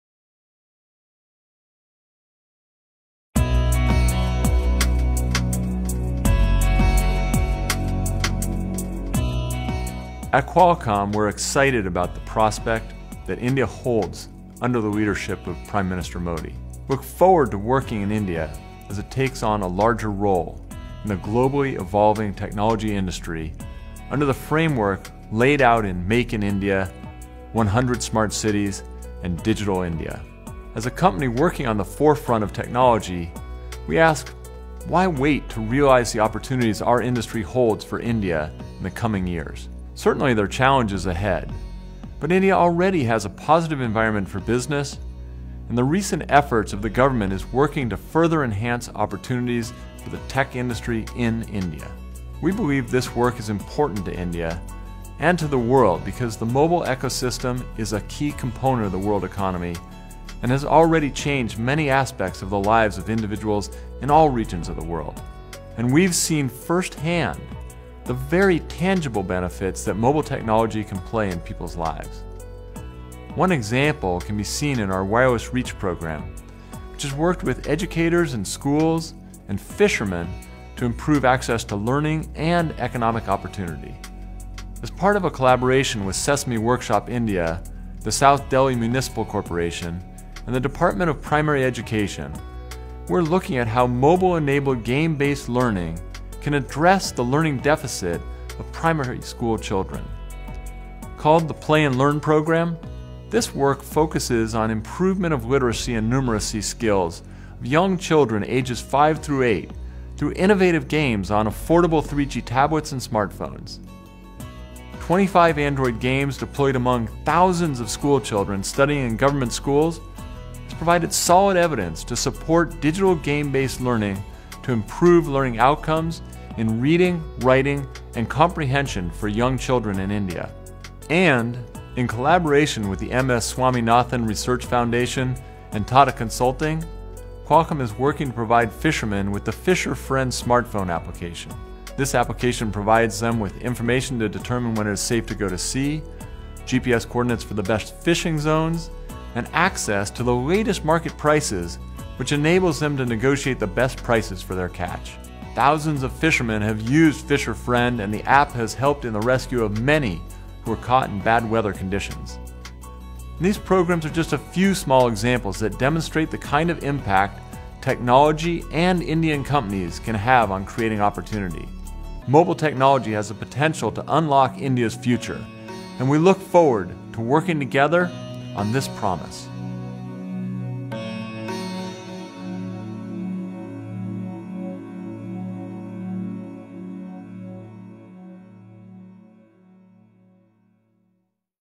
Dr Paul Jacobs , Executive Chairman, Qualcomm talks about PM Modi's Digital India Initiatives
"We are excited at the prospects that India holds under the leadership of Prime Minister Modi" Dr Paul Jacobs , Executive Chairman, Qualcomm talks about Prime Minister Narendra Modi's initiatives of Digital India, Make in India and Smart Cities.